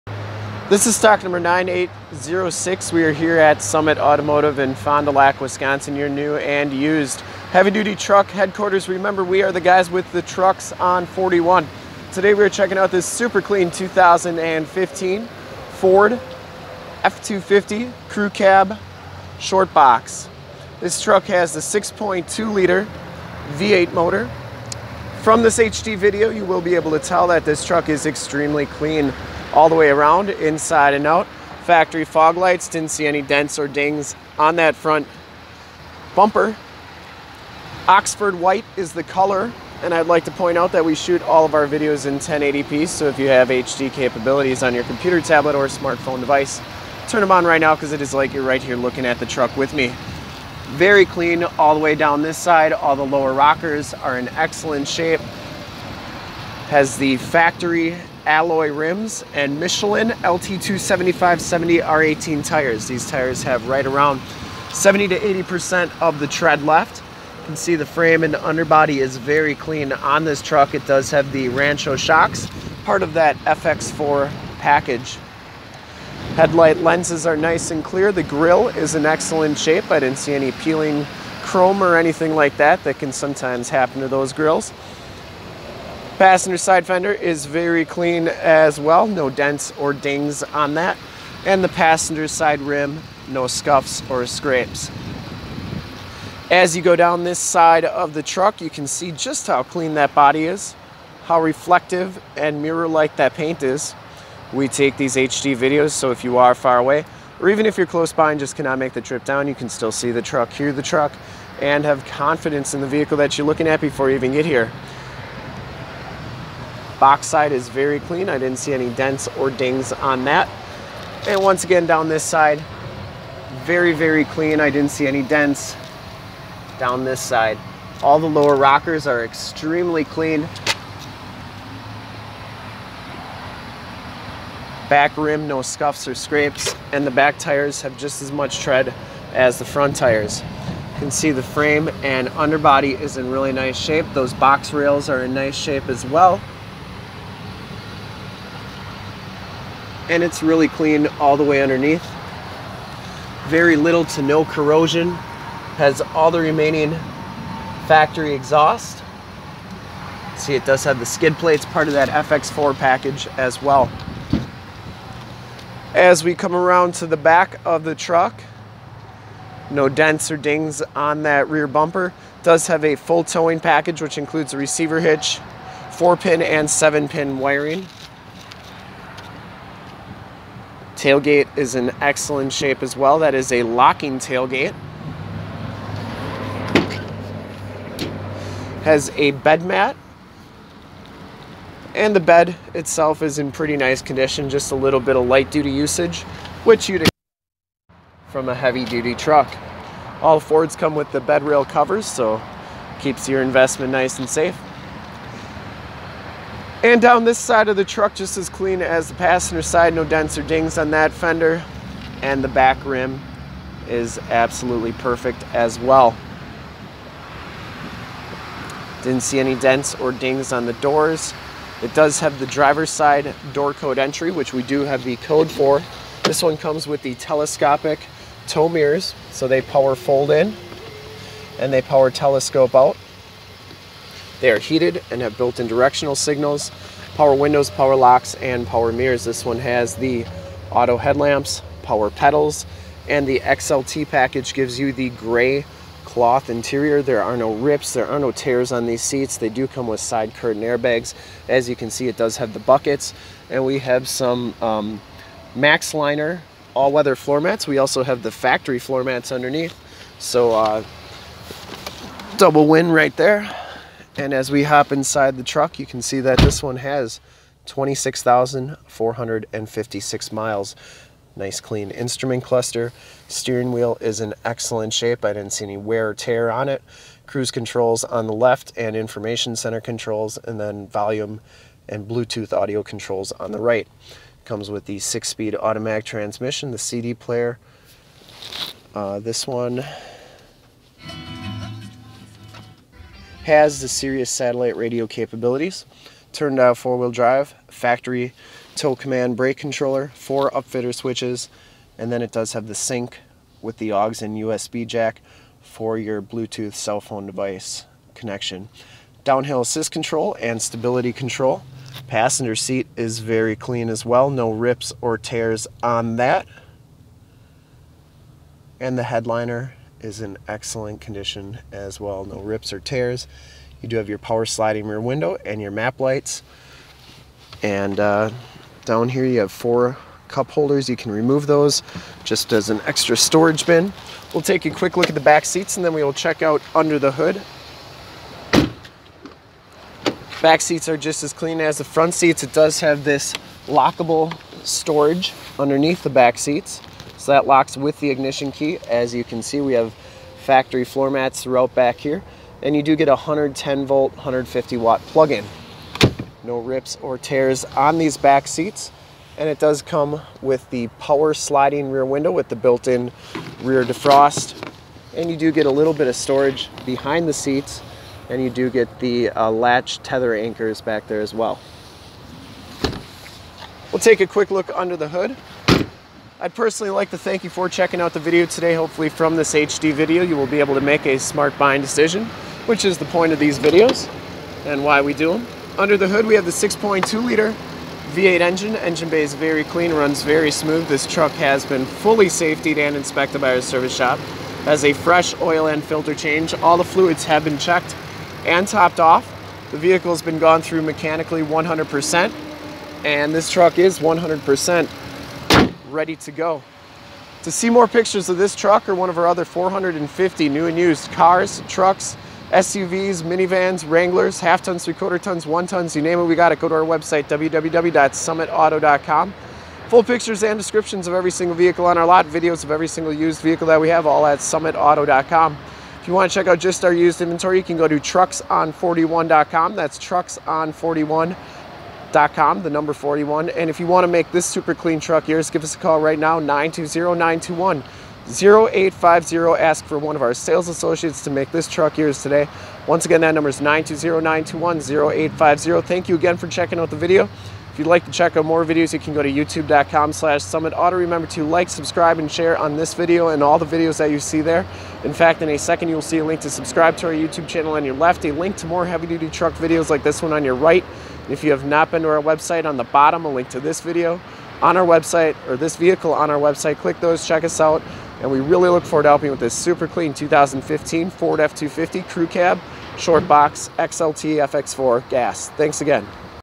2015 FORD F250 CREW SHORT XLT V8 GAS FX4 OXFORD WHITE WALKAROUND SOLD! 9806
STOCK:9806
PRICE: $33,999
MILES: 26,443
MAKE: FORD
MODEL: F250
VIN: 1FT7W2B63FED04526
LOCATION: FOND DU LAC OSHKOSH WISCONSIN, 54937 TRUCKS ON 41

1 OWNER! CLEAN TITLE HISTORY! 6.2 Liter V8 SOHC Engine with Flex fuel capabilities E85, 385 Horsepower, Full Four Door Crew Cab, Short Box 6 1/2 Foot Shortbox, XLT Package, 6 Speed Automatic Transmission with Optional Manual Tap Shift, Turn Dial 4x4 Four Wheel Drive 4WD, FX4 Offroad Suspension Package, Power Driver's Seat, Non Smoker, Gray Cloth Seats, Bucket Seats, Full Towing Package with Receiver Trailer Hitch, Wiring and Transmission Cooler Tow Package, 4 Upfitter Switches, Factory Brake Controller, Powerscope Tow Power Mirrors that Power Telescope and Power Fold in with Built-in Directional Signals, Advancetrac with Roll Stability Control Traction Control, HDC Hill Decent Control, 3.73 Gears wih Electronic Limited Slip Differential, Michelin LTX A/T LT275/70 R18 Tires, Bedrail Covers, Clearance Lights, Fog Lights, Raise Assist Tailgate, Reverse Sensors, Securicode Driver Side Doorcode Keyless Entry, Locking Tailgate, AM / FM Radio Tuner, Sirius/XM Satellite Radio Capabilities Sirius / XM, CD Player, Microsoft SYNC System with Bluetooth, Hands-Free Audio System Blue Tooth, Auxiliary MP3 Jack Portable Audio Connection, USB Jack Portable Audio Connection, Keyless Entry System, Power Sliding Rear Window Rear Window Defroster, Adjustable Height Seatbelts, Driver and Passenger Front Air Bags, L.A.T.C.H. Child Safety System, Side Curtain Air Bags SRS Safety Restraint System, Multi-Function Steering Wheel Controls, Compass, Outside Temperature Display and Mileage Display, Power Adjustable Pedals, Factory Floormats, Storage Compartment Under Rear Seats, Air Conditioning AC, Cruise Control, Power Locks, Power Windows, Automatic Headlights Autolamp, Tilt/Telescope Steering Wheel, 110V / 150W Auxiliary Power Outlet, 5 Year / 60,000 Mile Remaining Powertrain Factory Warranty, Whichever comes first, White, ONE OWNER! CLEAN AUTOCHECK! Very very clean inside and out! This is one of the sharpest 2015 Ford F250 crewcab shortbox 3/4 ton trucks we have ever had on our lot! Make your move before this super clean 4wd is gone!

STOCK:9806
PRICE: $33,999
MILES: 26,443
MAKE: FORD
MODEL: F250
VIN: 1FT7W2B63FED04526